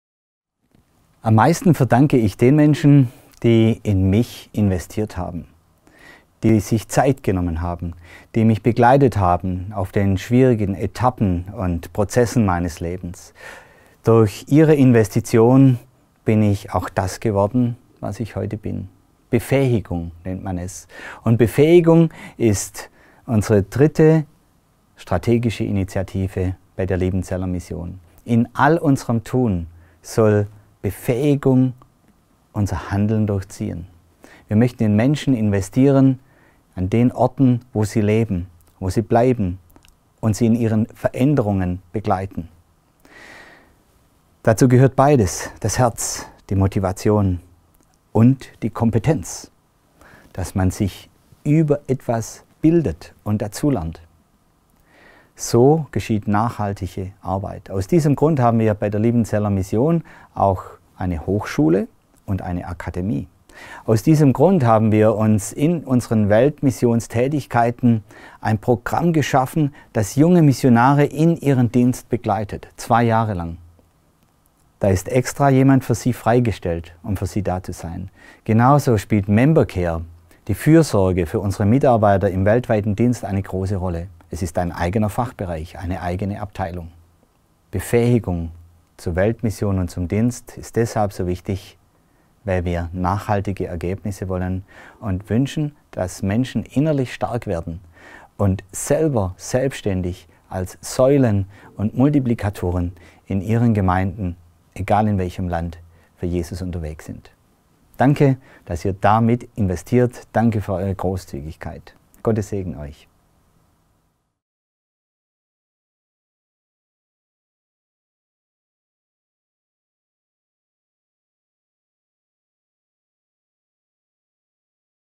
Martin Auch: Warum wir Mission fördern
Wir befähigen Menschen aus unterschiedlichen Kulturen, in ihrem Land und in aller Welt missionarisch zu wirken! Warum Mission heute nötiger ist als je zuvor, erklärt Missionsdirektor Martin Auch.